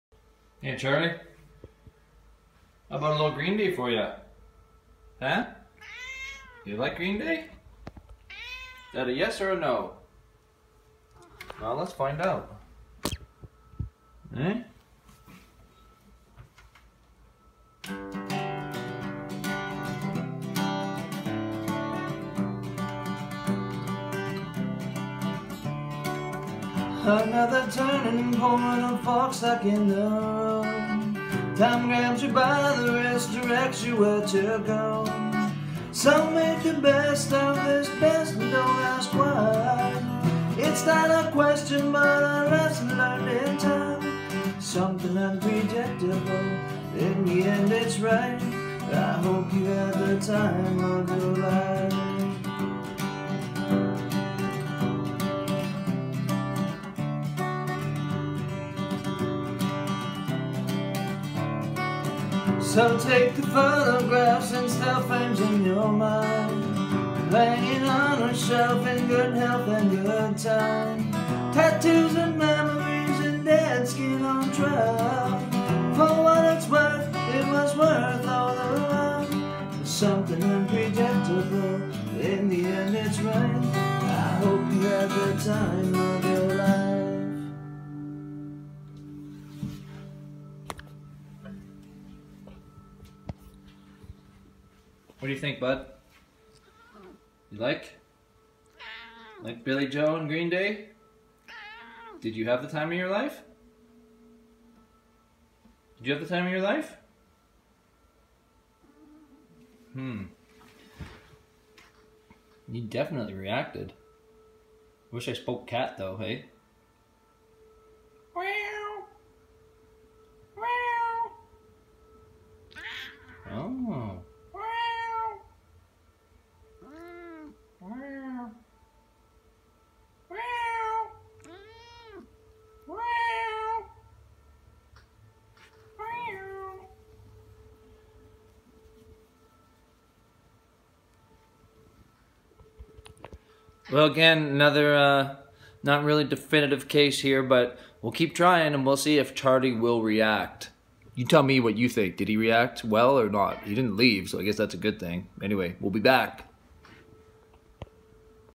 Will the Cat React? - Green Day Edition
Trying out a little Green Day on Charlie today.  Definitely got some reaction. He was very talkative today.  Still can’t tell if it was good or bad.  He didn’t leave at least so maybe he liked it?